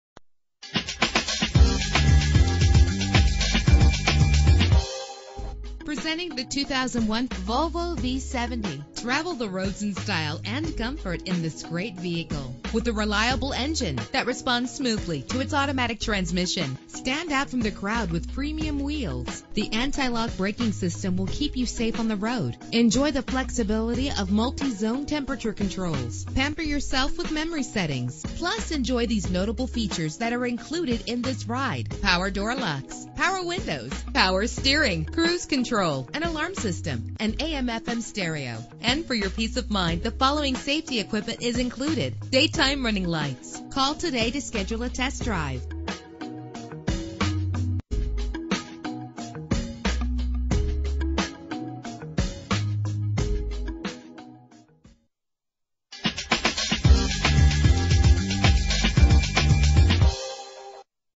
2001 Volvo V70 - Manheim PA
Year: 2001
Make: Volvo
Model: V70
Trim: XC AWD Wagon 4D
Engine: 5 cylinder turbo 2.4 liter
Transmission:
Color: Black
Mileage: 47823
Address:
14 Anthony Drive